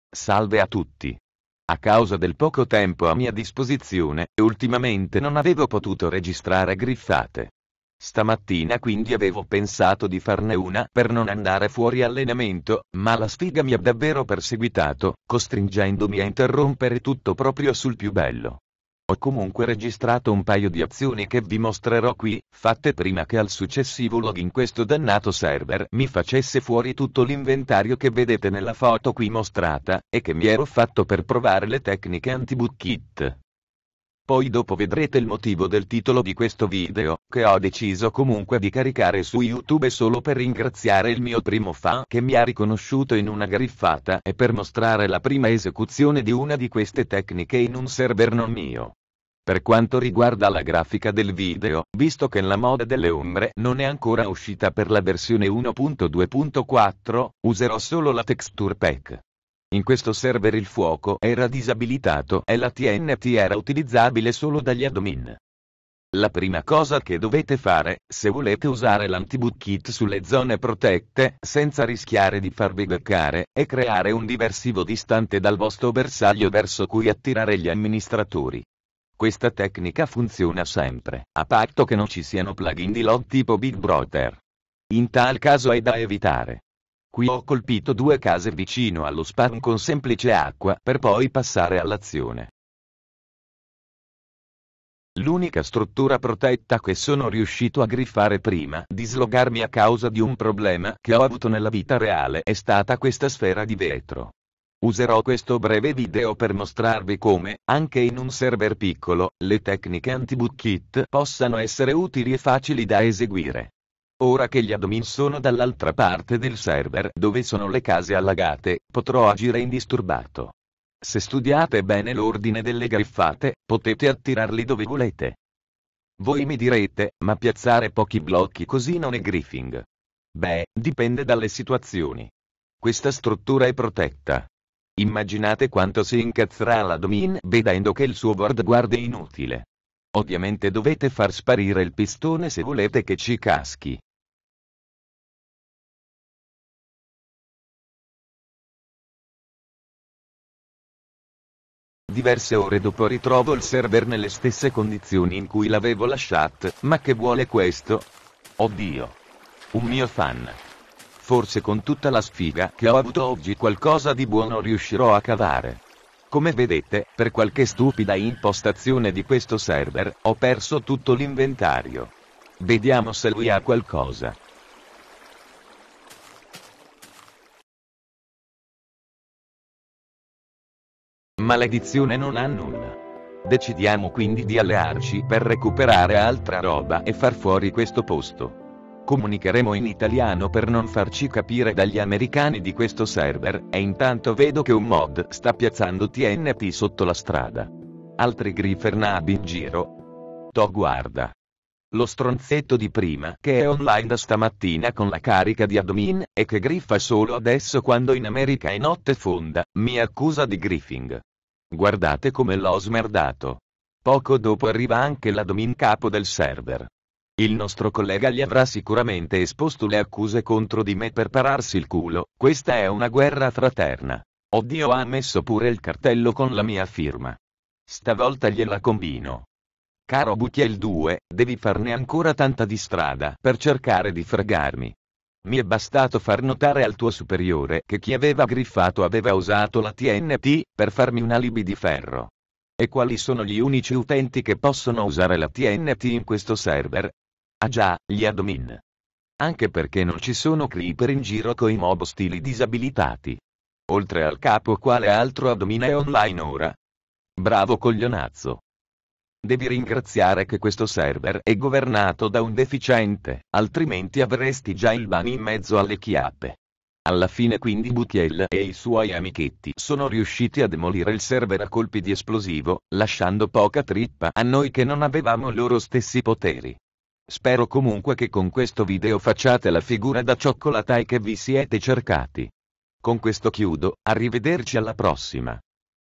[missione 5 1/2] Fraternal war
Non sono riuscito a fare una vera e propria griffata questa volta a causa di un problema improvviso e urgente a cui ho dovuto porre rimedio nella vita reale... ma ho deciso comunque di pubblicare il video: per festeggiare il primo fan che mi ha riconosciuto sul campo, per mostrarvi una delle mie nuove tecniche in funzione su un server non mio... e per smerdare un griefer nabbo che ha provato a farmi bannare! :D